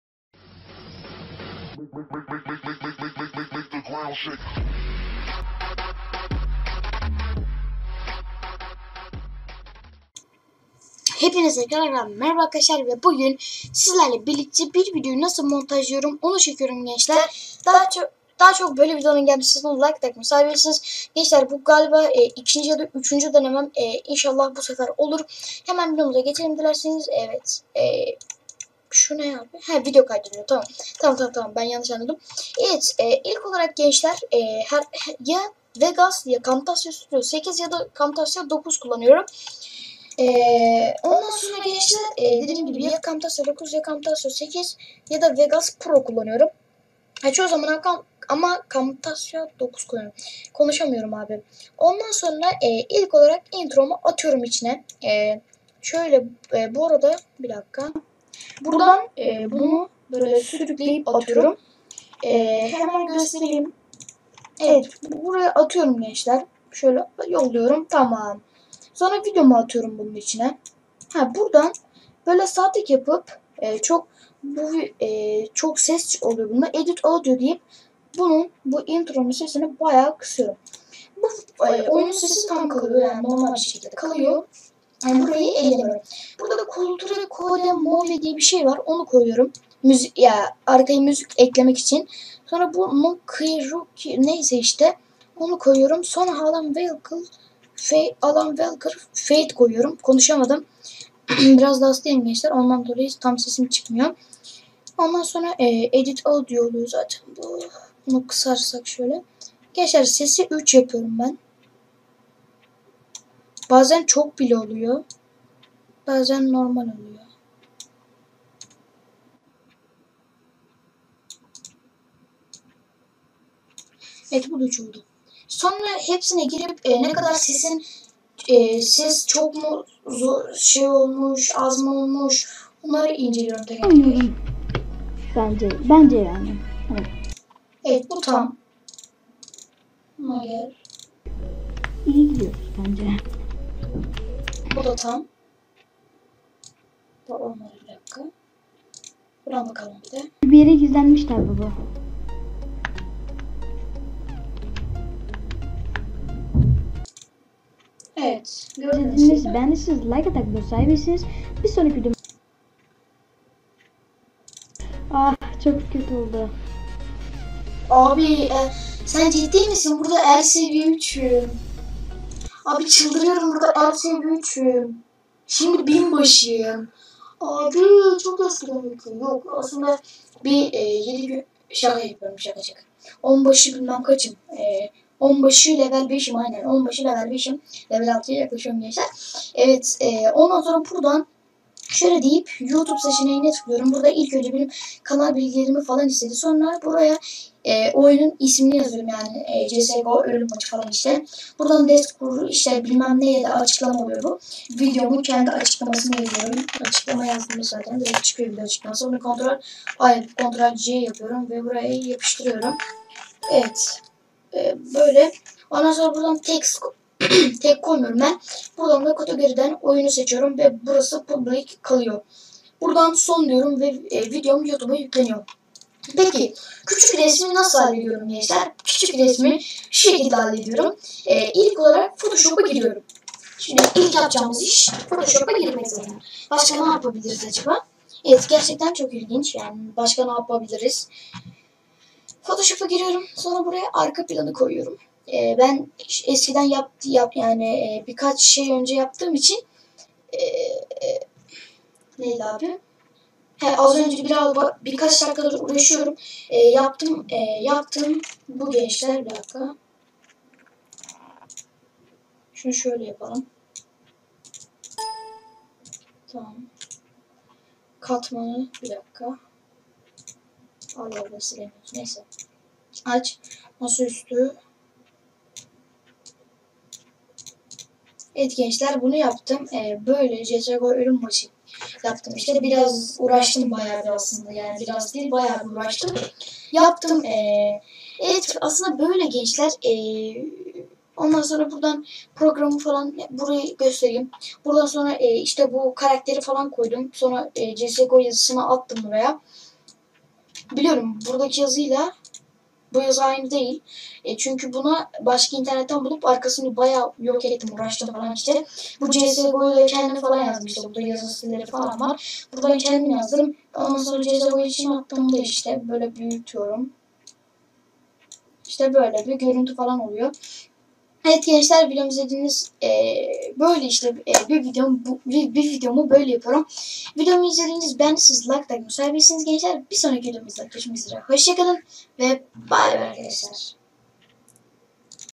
Bir Videomu Nasıl Yapıyorum ! | Küçük Resim , Montaj , Müzik Ekleme ... !!!!
İyi Seyirler

İninal Barkod : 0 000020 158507
Bu Seri Sizi Desteklemek İçindir

videoma gelmek için bağış atmanız yeterli bağış fiyatı 2 tl